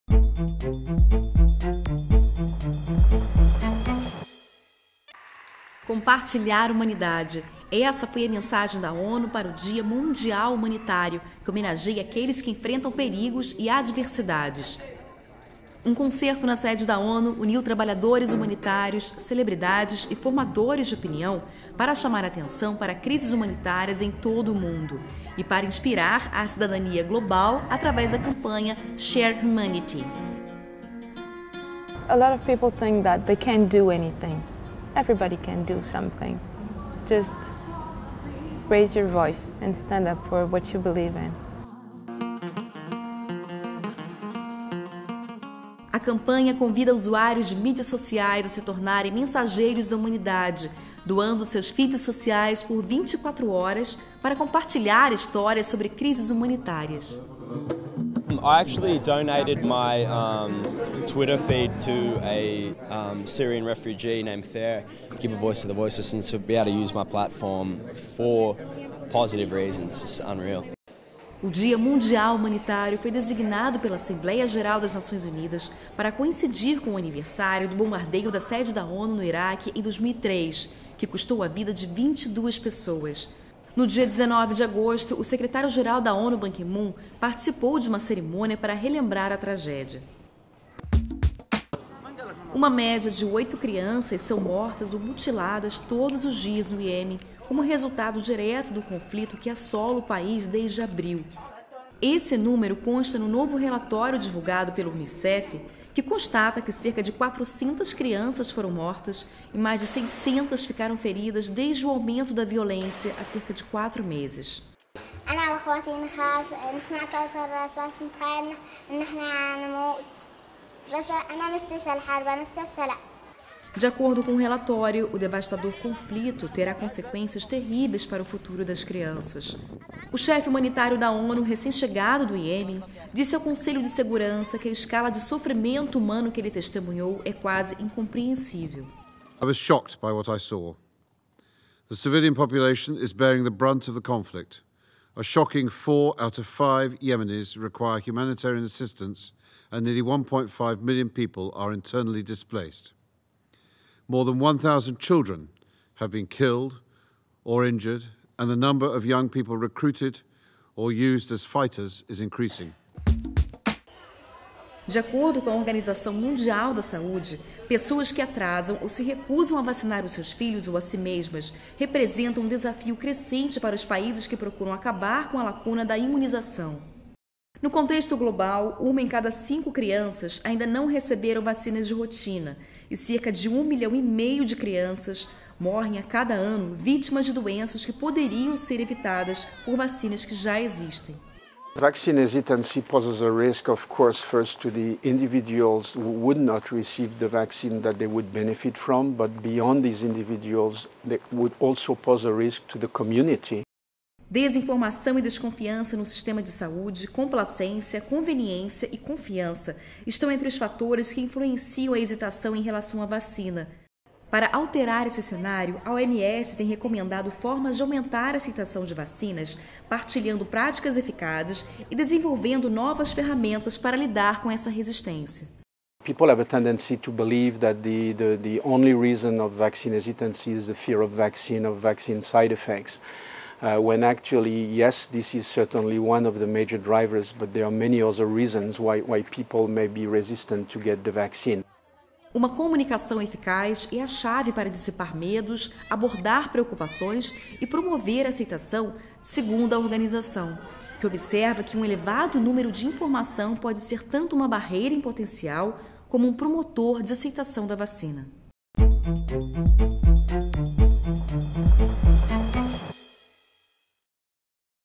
Resumo semanal da ONU em imagens #56
ONU promove o Dia Mundial Humanitário em concerto com trabalhadores humanitários e celebridades musicais; novo relatório do UNICEF alerta para o número de crianças vitimadas no conflito no Iêmen; OMS aborda o desafio em acabar com a resistência à vacinação - estes são os destaques do resumo semanal da ONU em imagens.